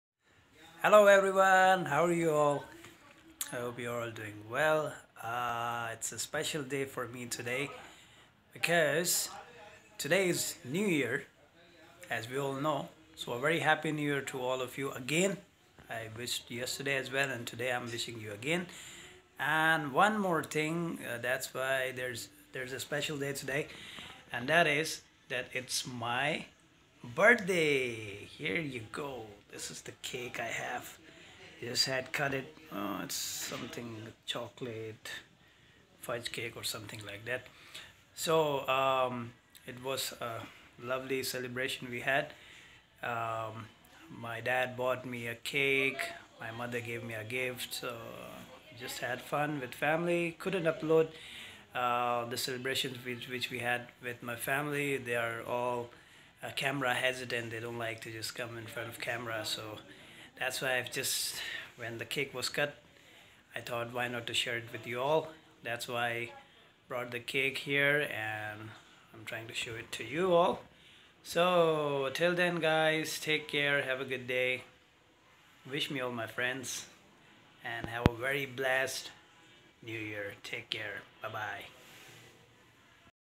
It's my birthday on New year. How lucky I am MashAllah
1 Jan 1992 I was born. Turned 28 today, Alhamdulillah